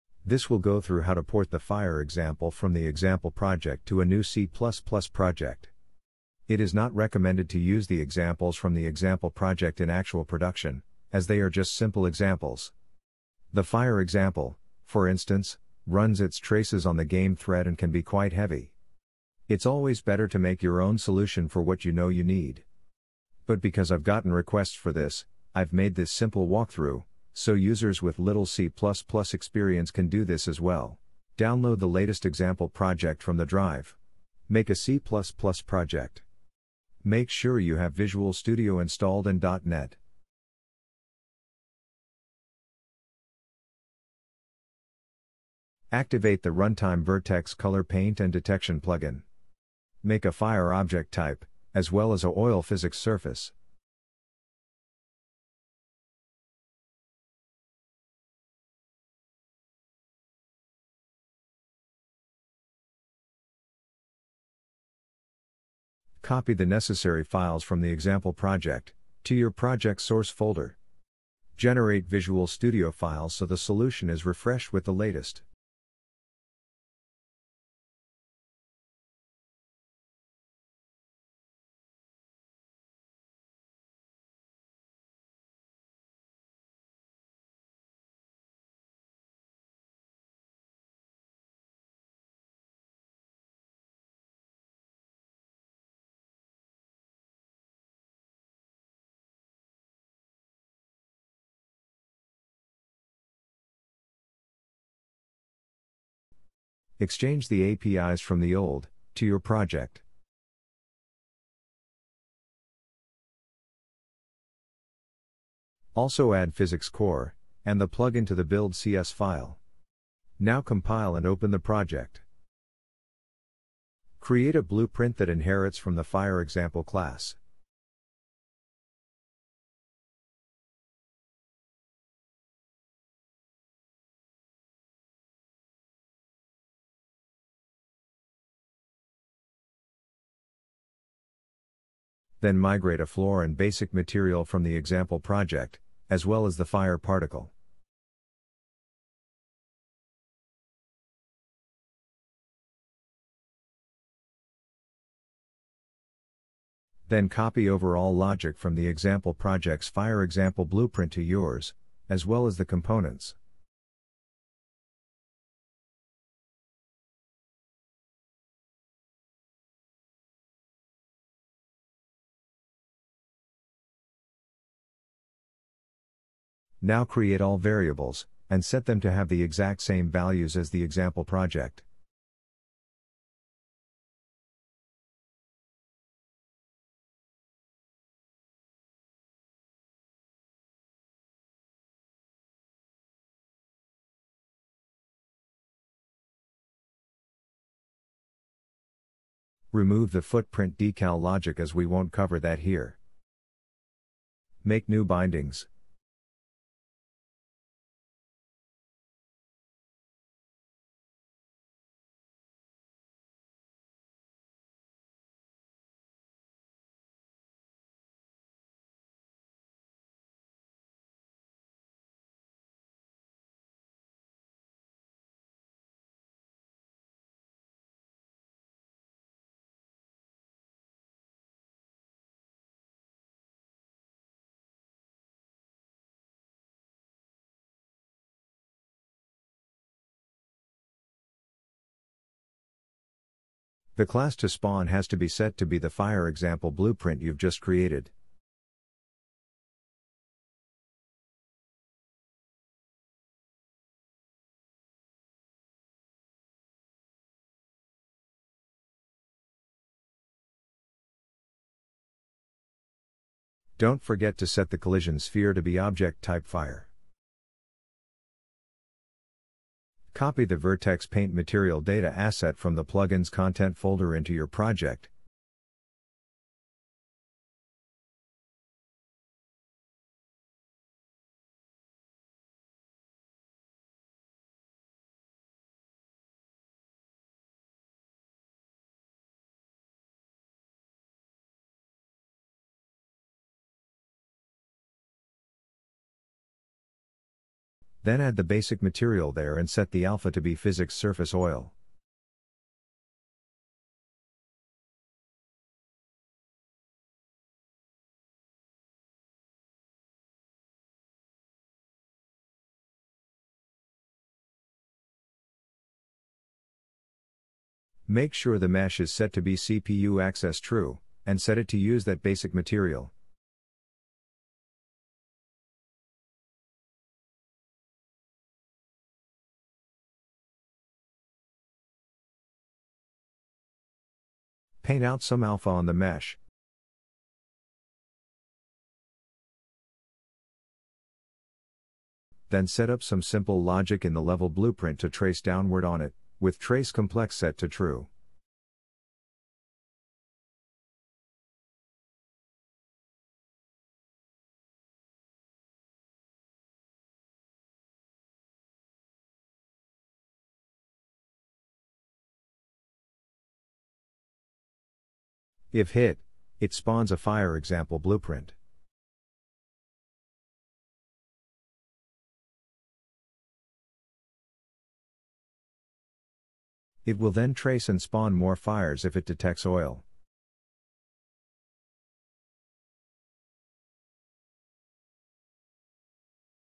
Porting Fire Example Tutorial - Runtime Vertex Color Paint & Detection Plugin
This is a short walkthrough how to port the Fire Example from the Example Project to a new C++ Project.
NOTE That in the latest example we've made a minor change in BP where we instead of running Paint at Location on the mesh the fire got spawned on, we run it on all Overlapping components of the FlameCollisionSphere. This requires the meshes it should remove oil from to generate overlap events. You can check it out in the latest example project.

NOTE That it is not recommended to use the examples from the example project but make your own that's suited for your needs. The fire example while it may like nice, is quite heavy as it runs it's traces on the game thread.

Paint, Remove & Detect Vertex Colors in Runtime to enable extreme attention to detail and new Game Mode and Gameplay possibilities!